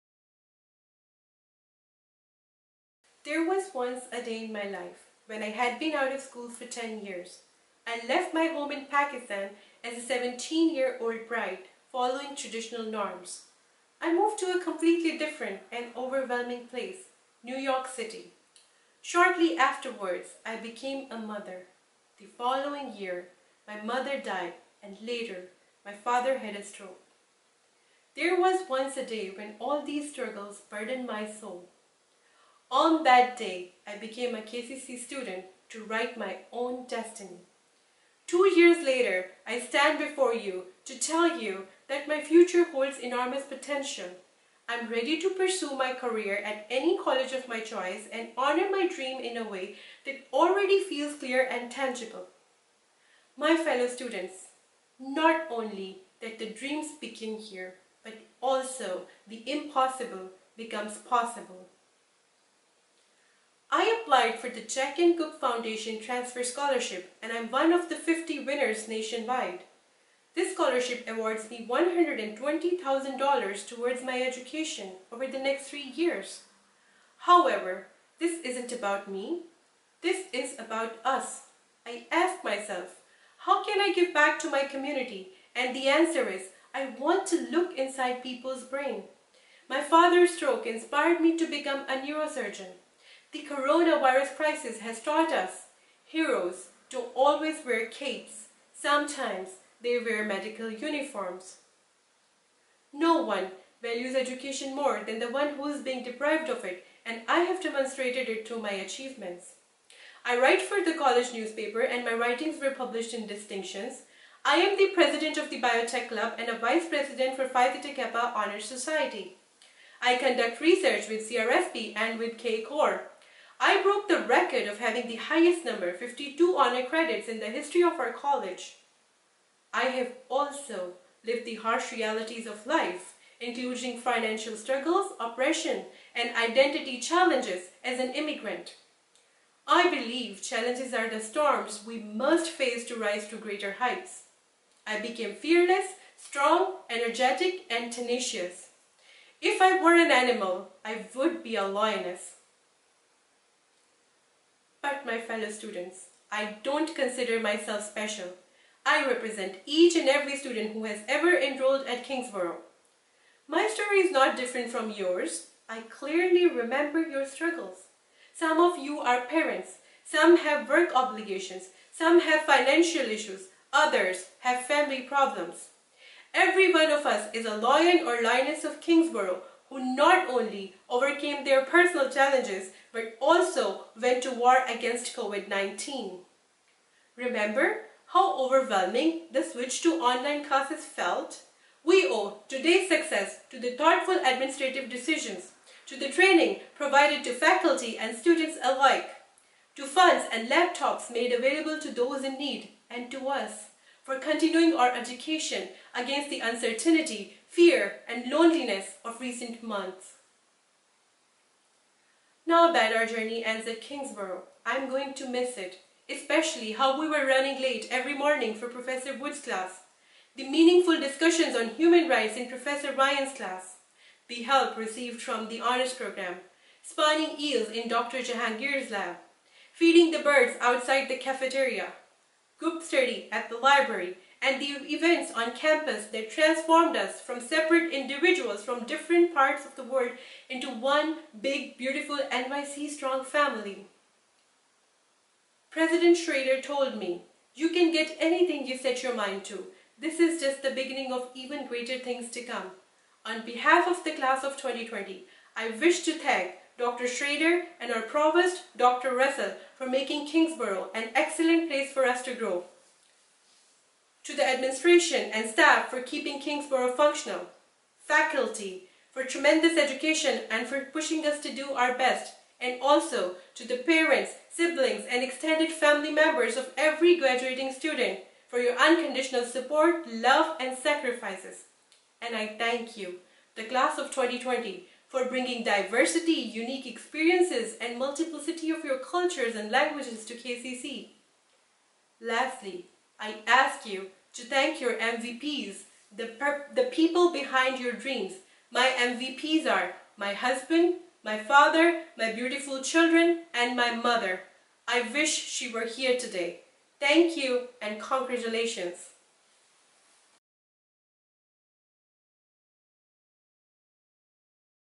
Valedictorian JAWERIA BAKAR
Valedictorian Jaweria Bakar powerful message to the graduating class of 2020.